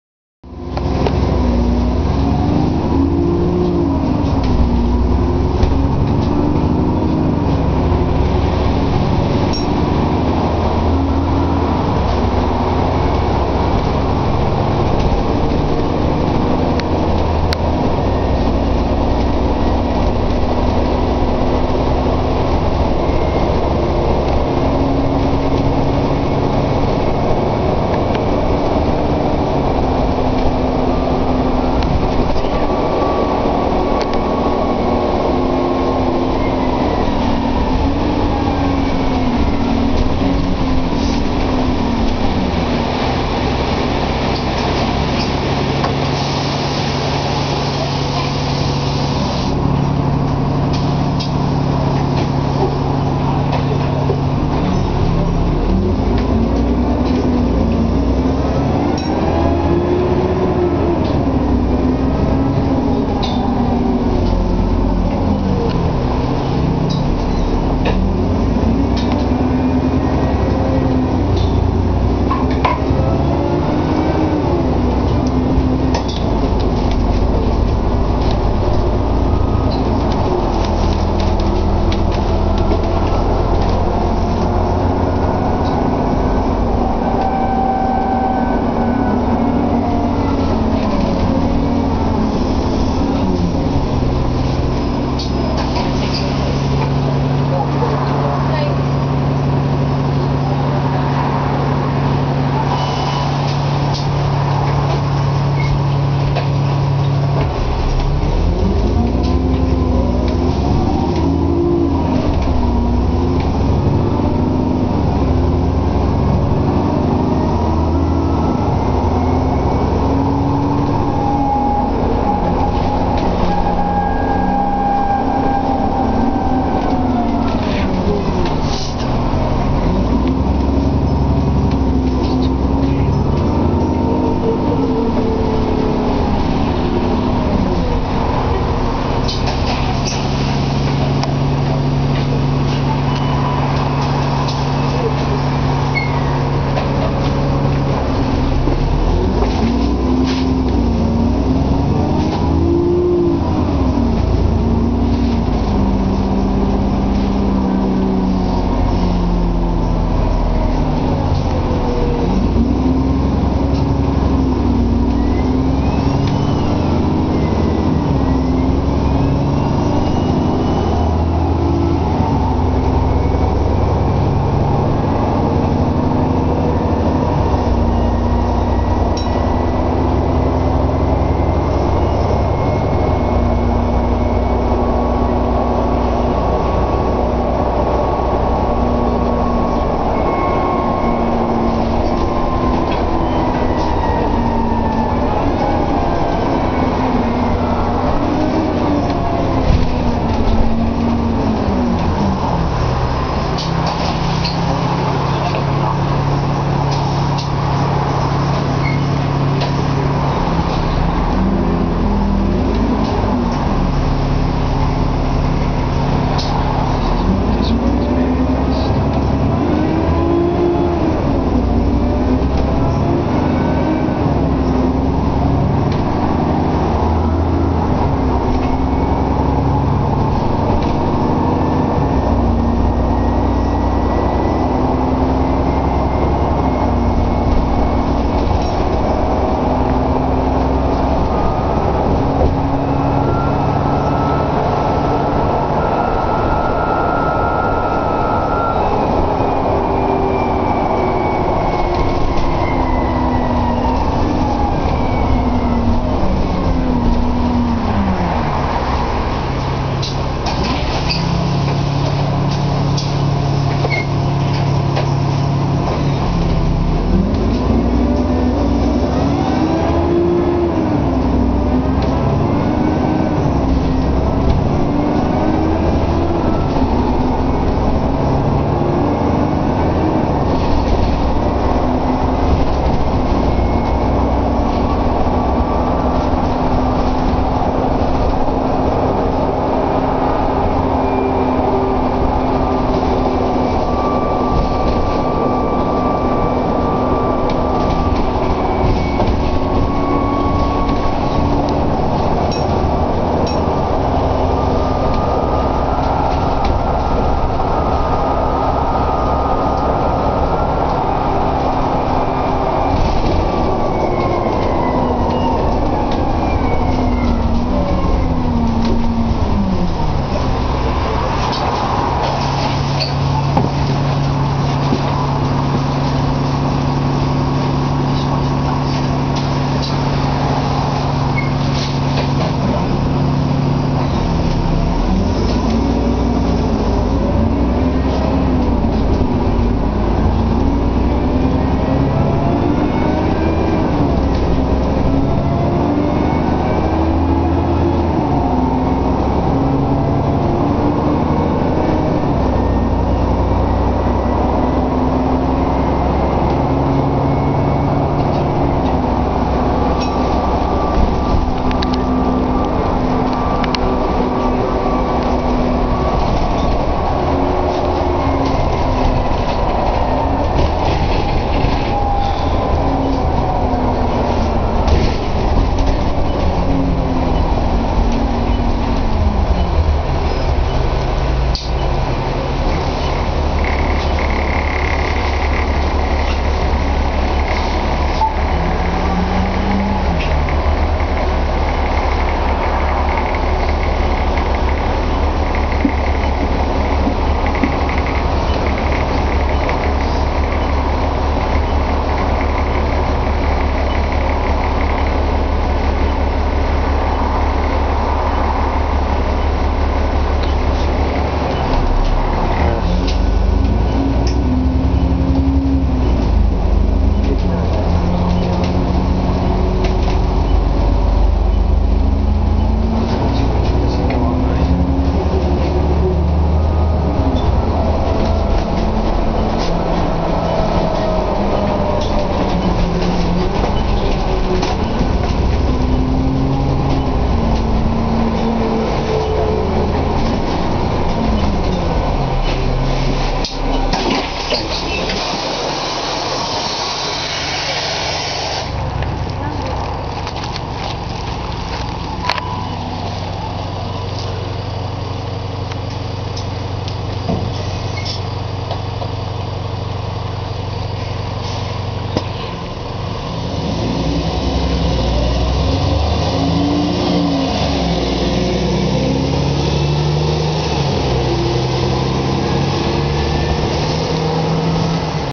NX West Midlands | Alexander Dennis Enviro 400 | 4941 (SL14LRK) | Route 957 | 09/08/2014
NEW National Express West Midlands Dennis Enviro 400 4941 operating the 18:15 957 from Birmingham to Solihull filmed here from The Bill & Bull Pub, Yardley to Sheldon, Old Lode Lane (My Local Stop for the 957 to Solihull). 4941 is currently based at Birmingham Central Garage